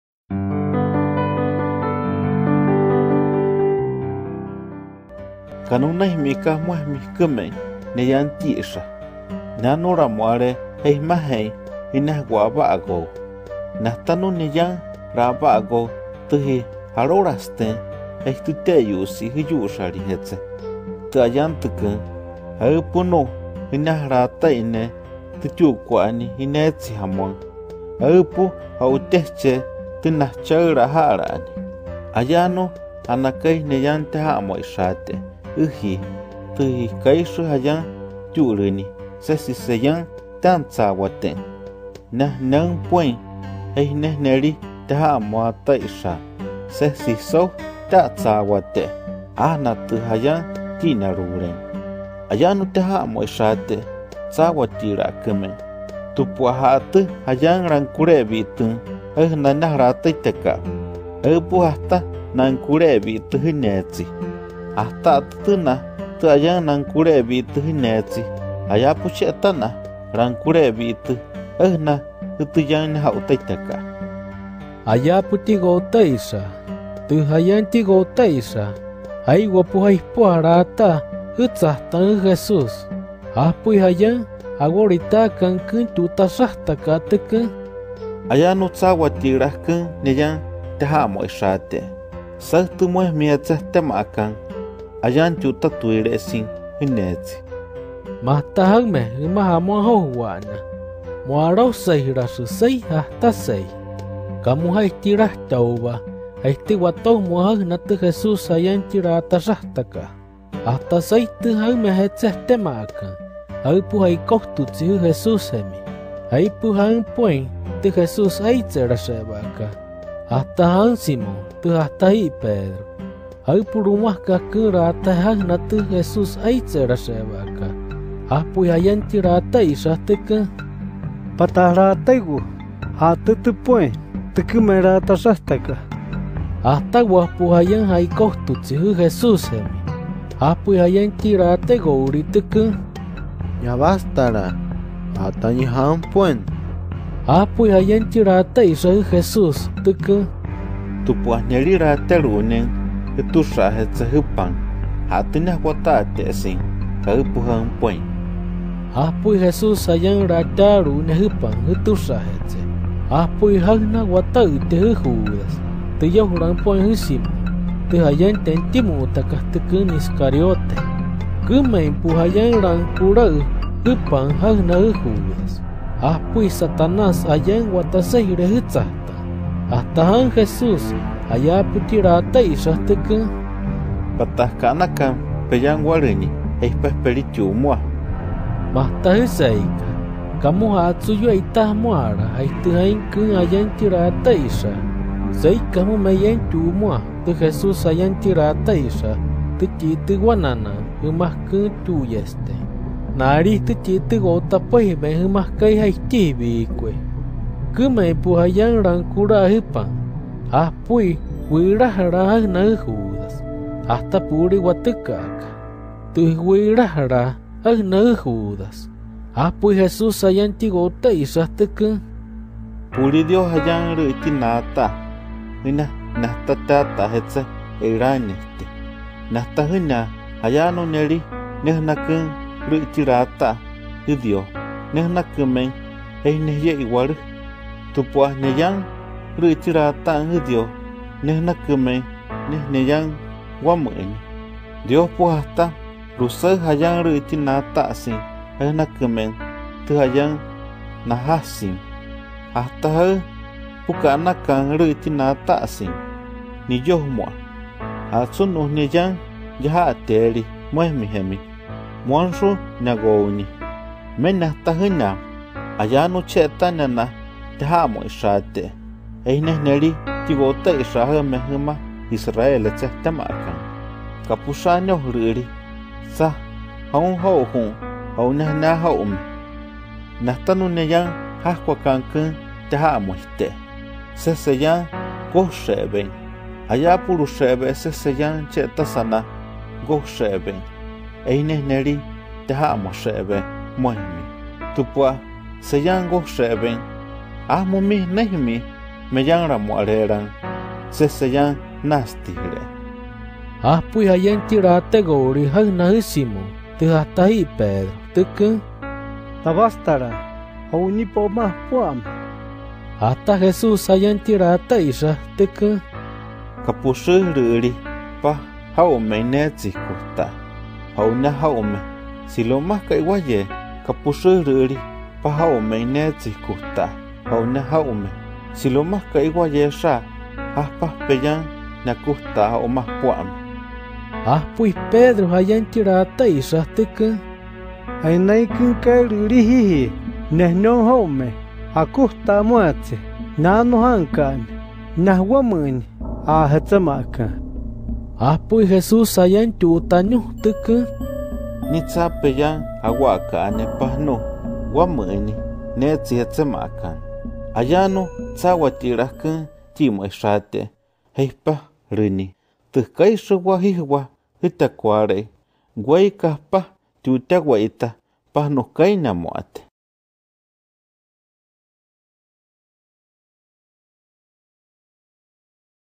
Cora, El Nayer - John 13:18-38 - Jesus predicts betrayal [crn]
Selection from the Gospel of John in the Cora, El Nayer language of Mexico [crn]